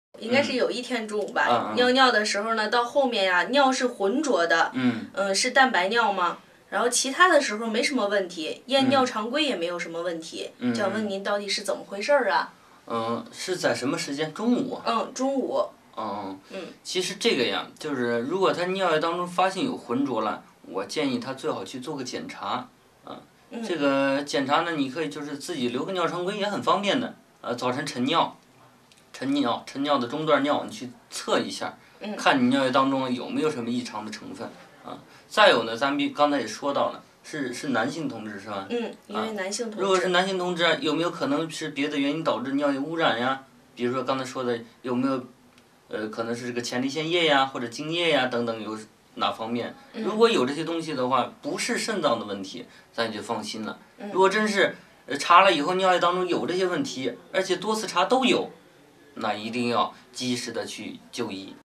I pee at noon one day, the urine is also muddy. Is it proteinuria?
There is no problems at other time and urine routine is normal.
What is the matter, please?
What is the time? At noon
Yes  it is at the noon
If fact, there is muddy in the urine
I suggest he had better make a check
It is also convenient to take a urine routine
He uses the medistream urine of morning urine to check
See that whether there is abnormal compositionin in the urine
And we just mentioned it just now
It’s a male comrade
If it is a male comrade, whether it is possible that other urine can cause pollution
For example, has it been possible that the prostate fluid or semen and so on?
If he has these things, it is not problem of kidney.we can rest assured
But there are these questions in the urine after checking
And it has problems after many examinations
Must see a doctor in time